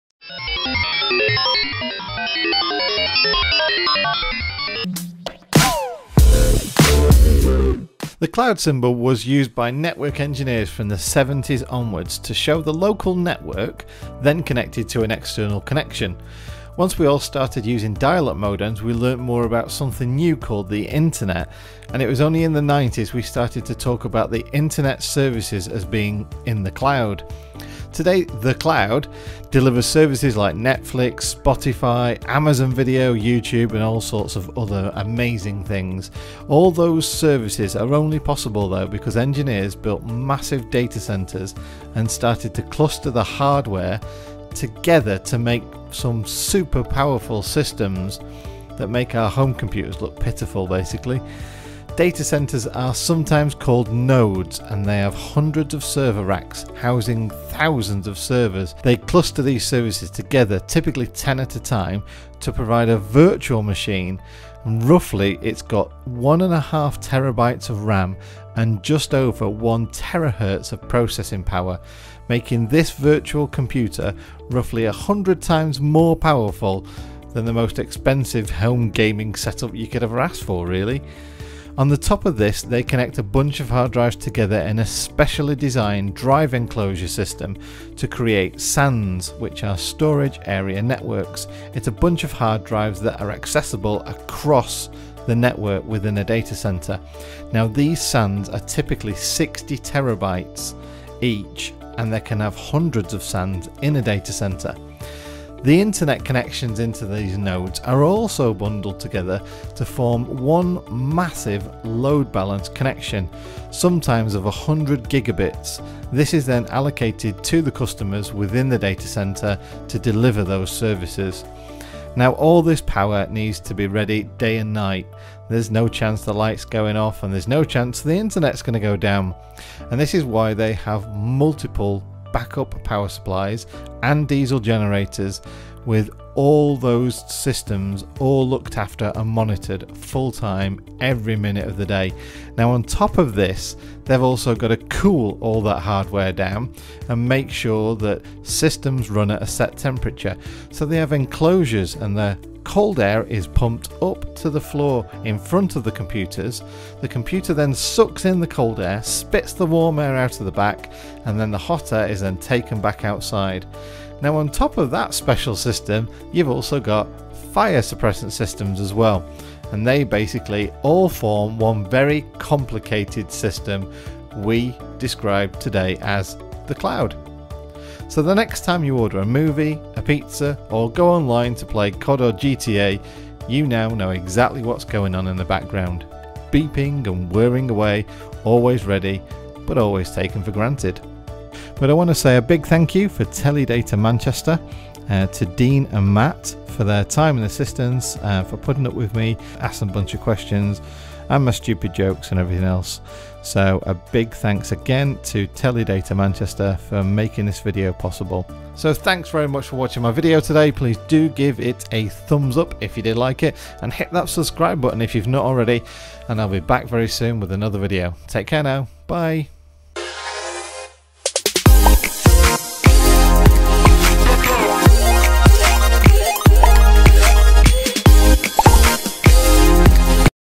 What is The Cloud - A Simple Explanation
Ever wondered how Netflix or YouTube can handle all these users and all the data? Let me give you a tour of a data centre and explain.

Thanks to Teledata Manchester for their help in making this video.

All views are my own, are not to be taken personally and not to be trolled, cos that's just not cricket folks!